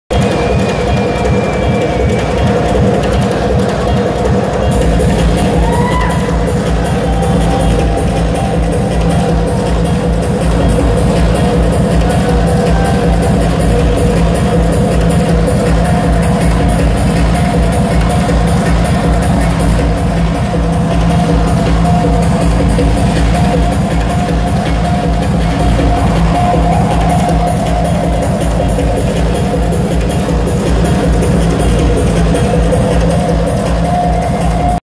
Format C:\ Live machines @ X-Massacre 2018 / Expo Hall Prague / CZ (PART 2)
Format C:\ Live machines.
Gear used: Yamaha RS 7000, xOxbOx 303, Korg Electribe ER 1, Korg Electribe EMX 1, electro-harmonix BIG MUFF deluxe, div. fx.